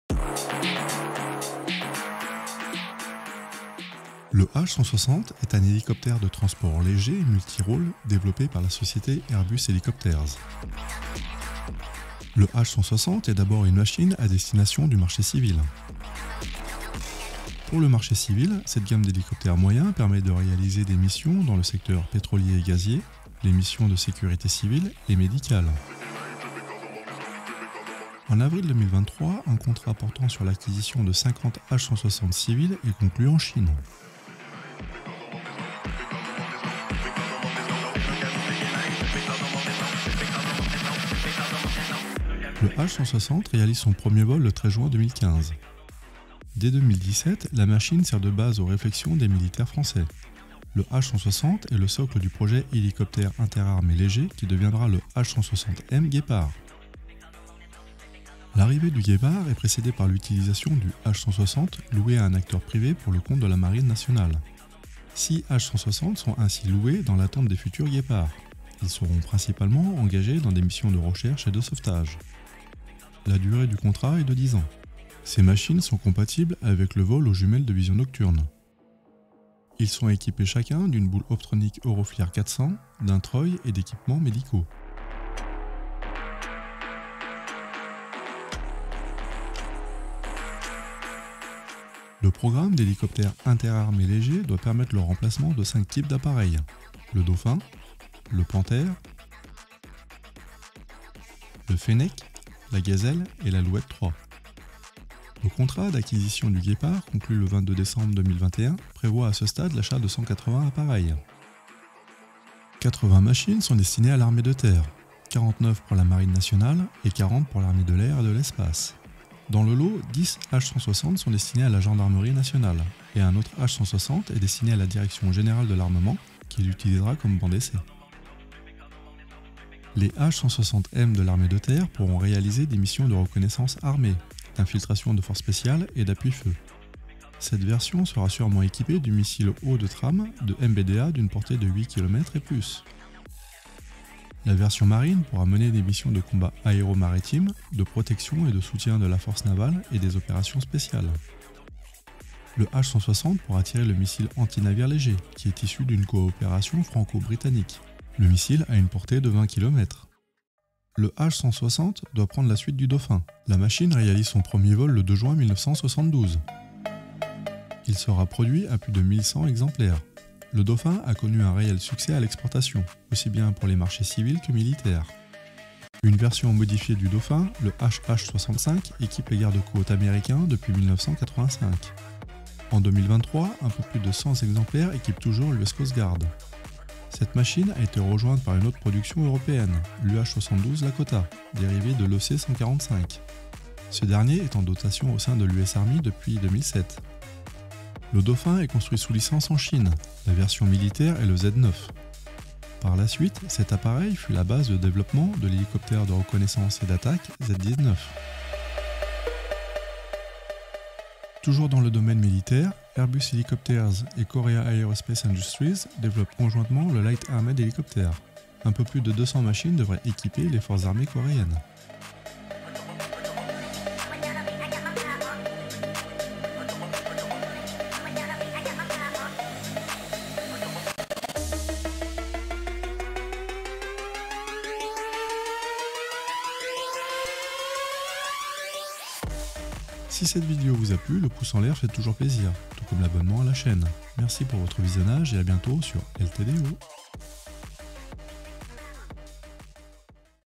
Hélicoptère H160
Le H160 est un hélicoptère de transport léger multirôle développé par la société Airbus Helicopters. Le H160 est d'abord une machine à destination du marché civile. Pour le marché civil, cette gamme d’hélicoptères moyens permet de réaliser des missions dans le secteur pétrolier et gazier, les missions de sécurité civils et médicales. En avril 2023, un contrat portant sur l'acquisition de 50 H160 civil est conclu en Chine.

A bientôt,

LTDO